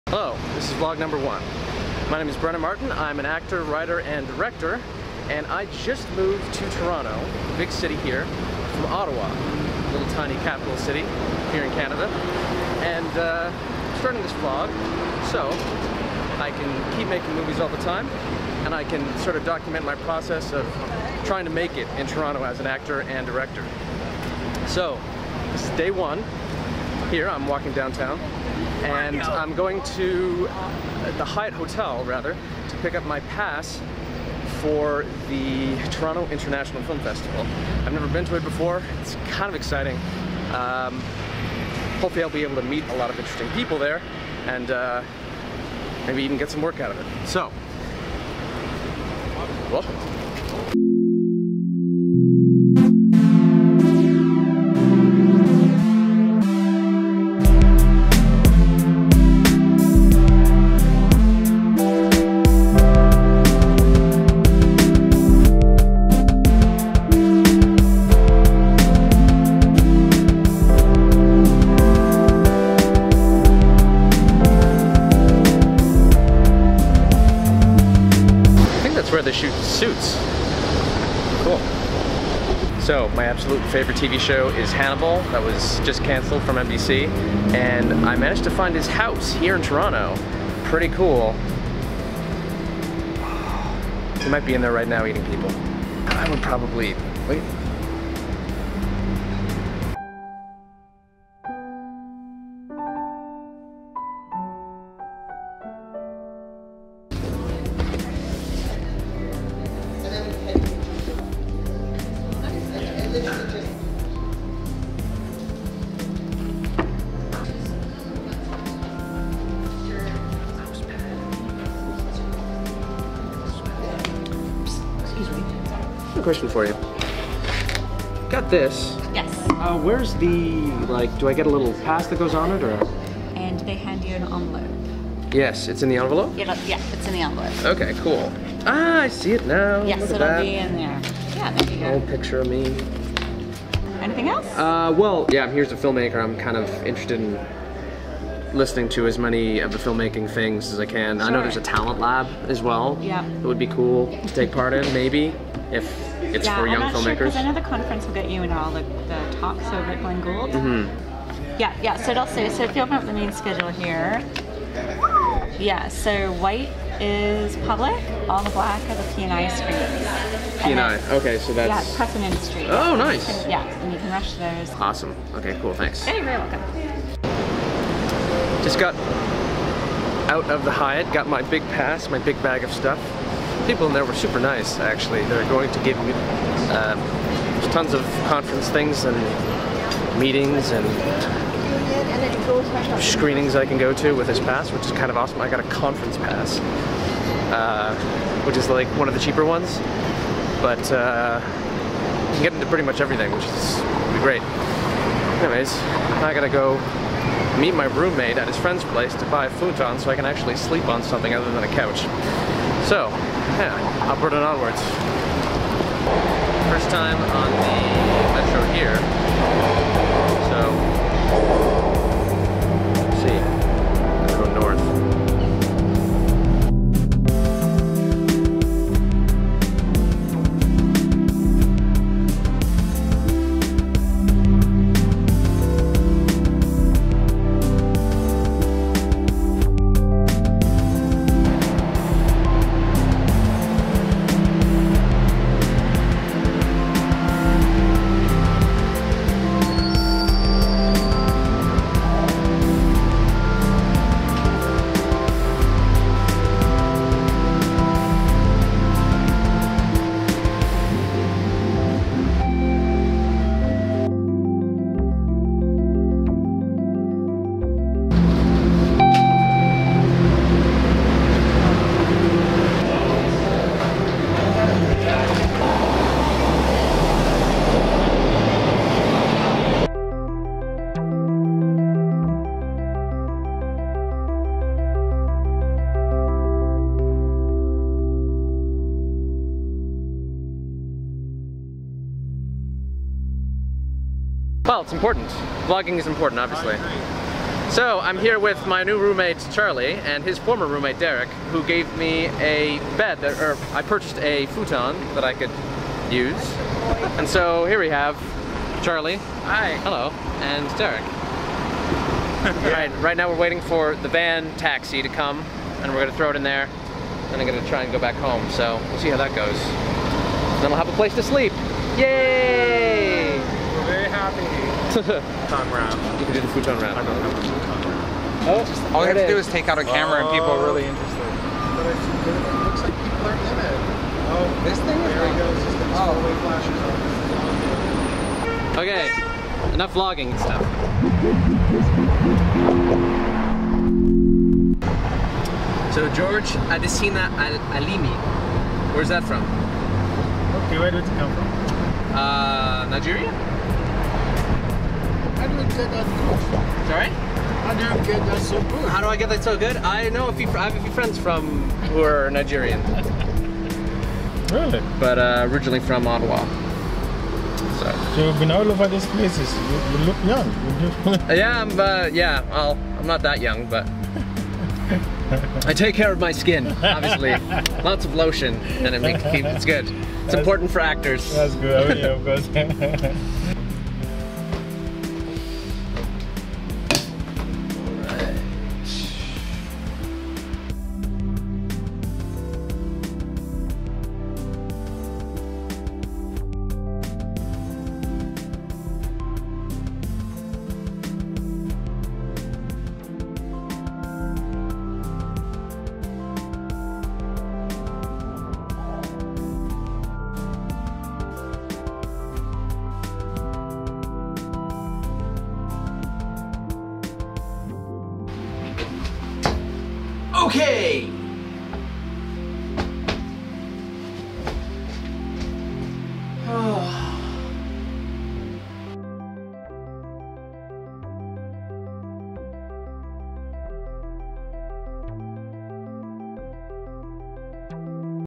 Brennan's First Vlog September 8th, 2015
My first attempt at vlogging, taking a lot from Casey Neistat as inspiration. Music by Half-Jay